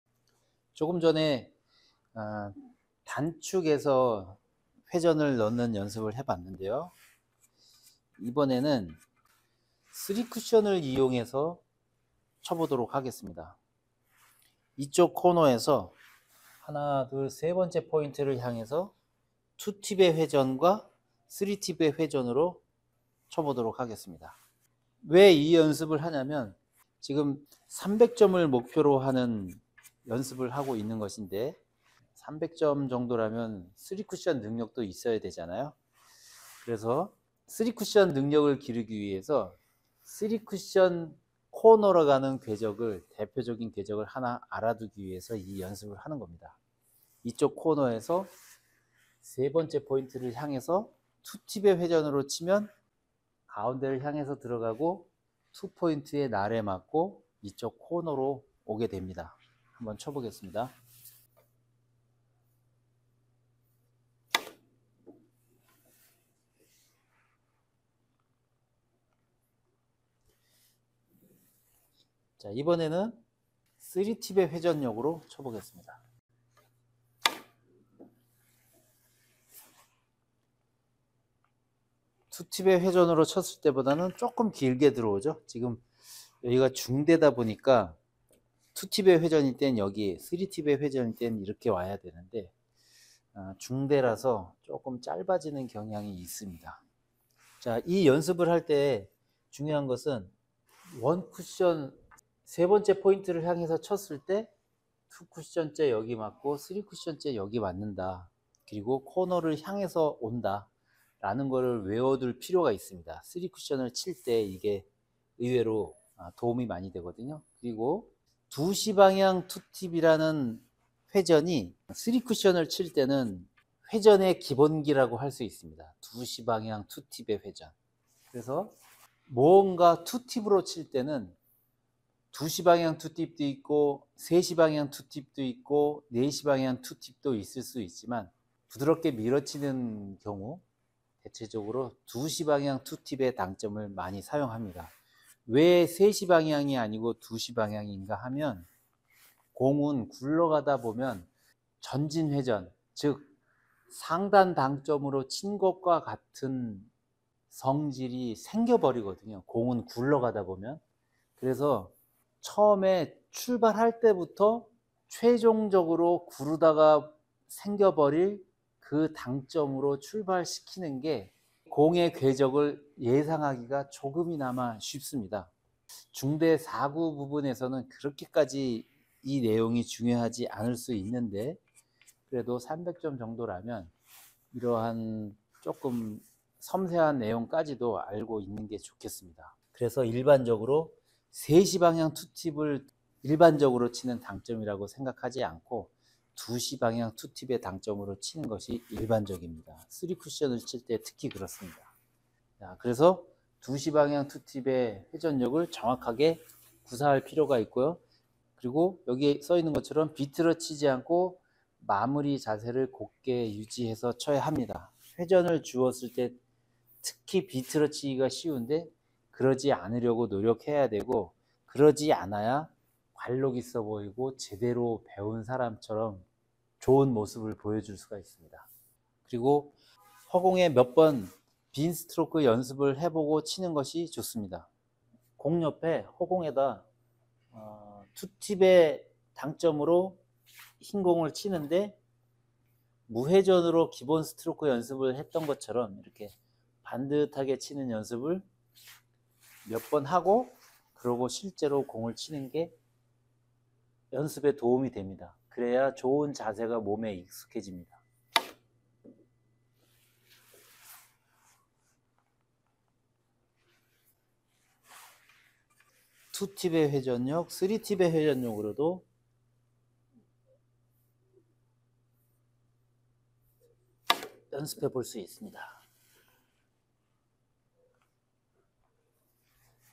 300점 만들기 프로젝트 2a 기본 3쿠션라인 회전연습
300점 만들기 프로젝트 2a는 기본 3쿠션라인 (코너출발 반대편 코너로 보내는 라인)을 일반적인 회전(2팁)으로 치는 연습입니다.

당구박사앱 '300점 만들기 프로젝트'에 수록되어 있습니다.